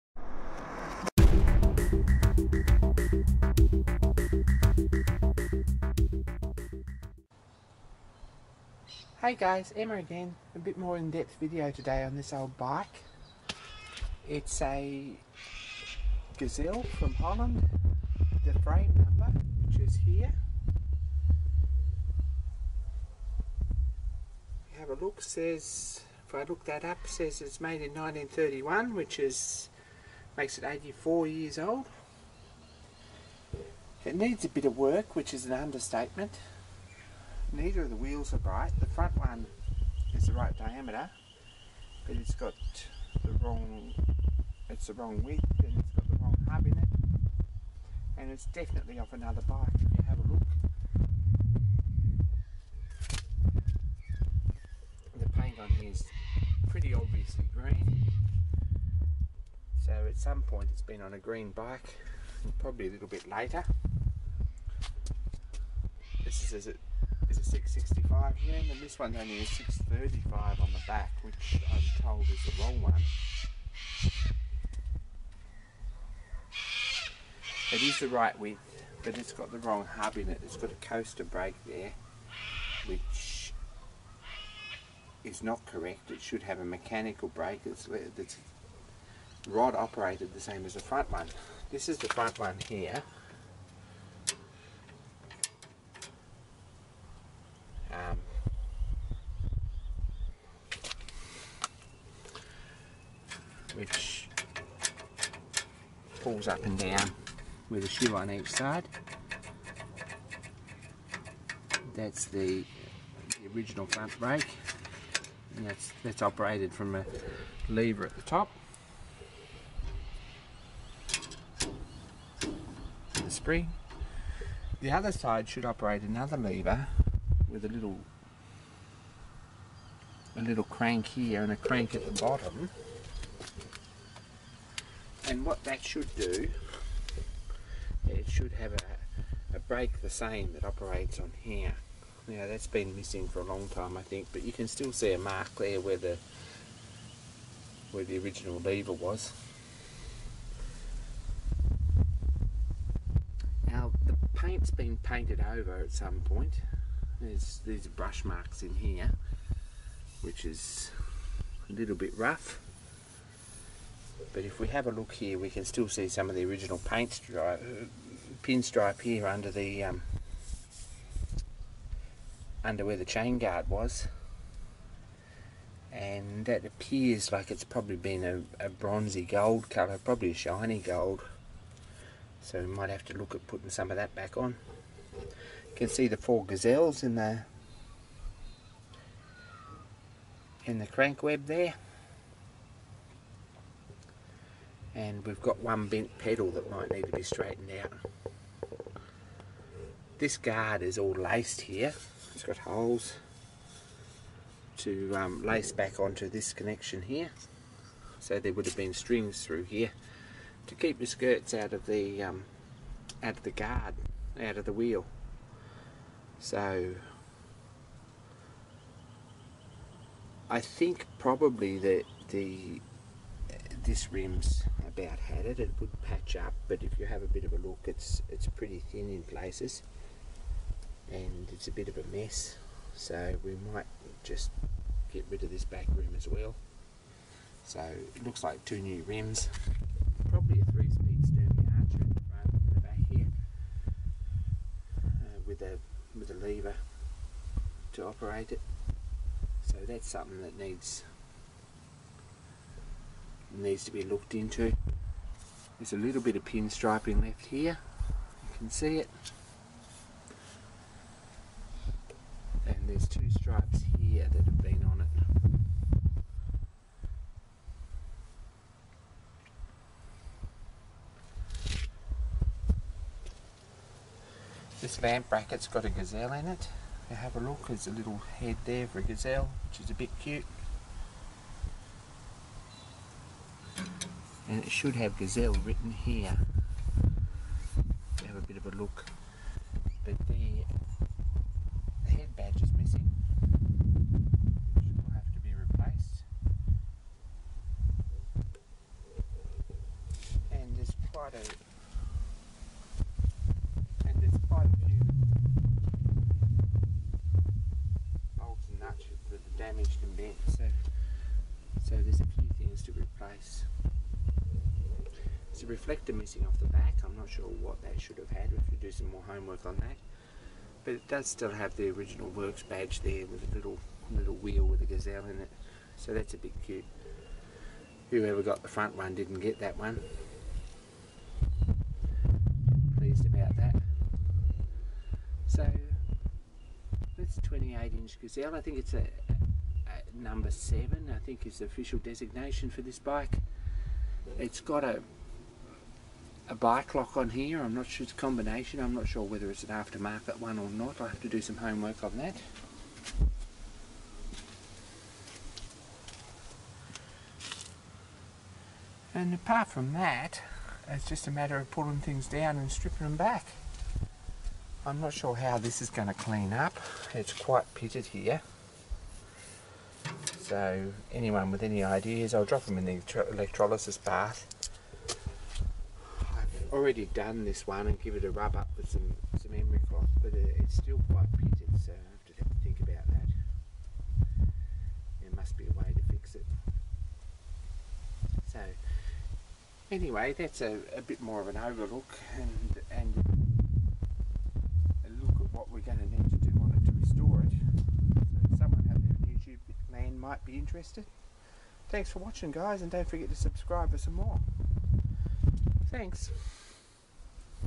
1931 Gazelle Bike a closer Look
taking a closer look at my 1931 Gazelle ladies touring bike, and the work needed to restore it.